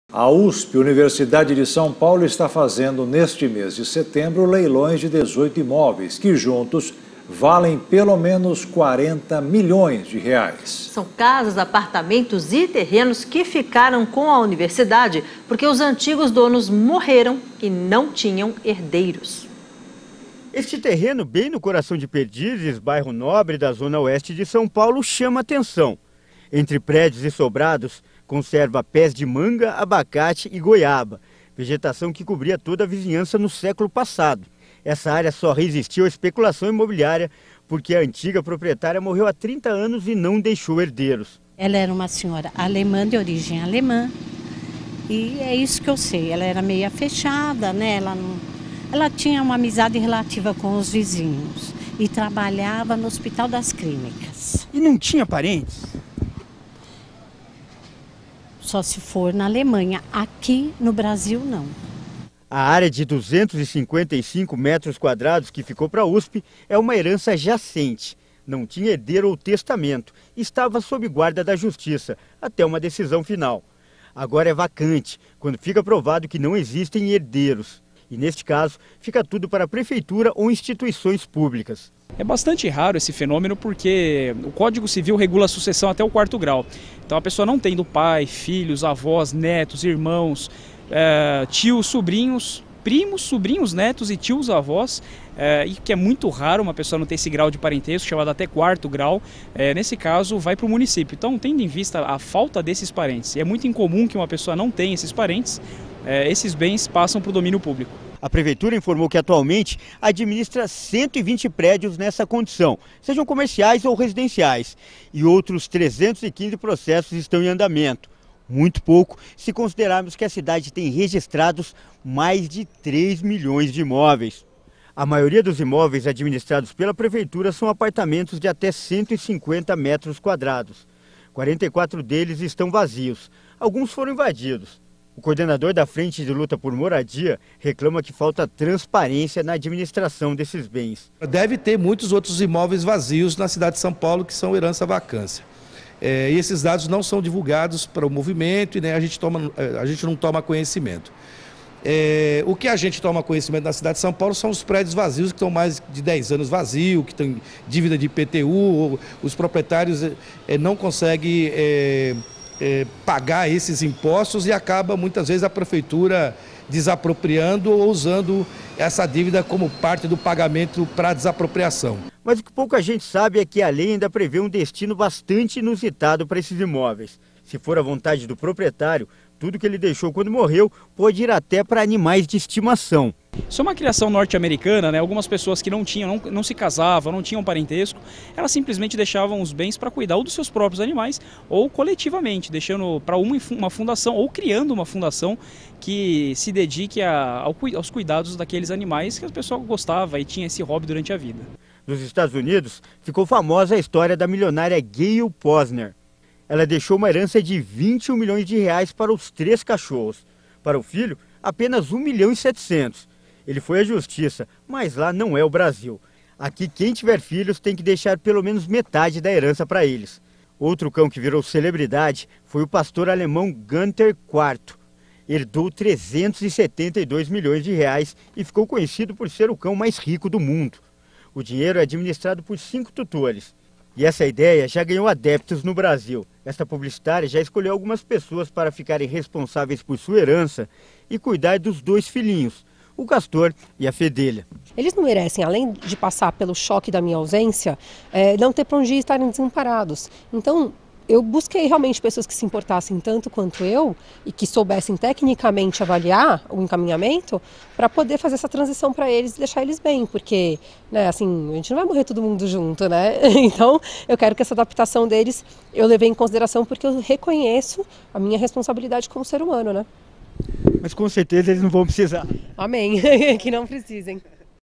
Heranca jacente e heranca vacante: USP promove leilão de bens avaliados em 40 milhões de reais
A Universidade de São Paulo, está fazendo, neste mês de setembro, leilões de 18 imóveis que, juntos, valem pelo menos 40 milhões de reais. São casas, apartamentos e terrenos que ficaram com a universidade, porque os antigos donos morreram e não tinham herdeiros.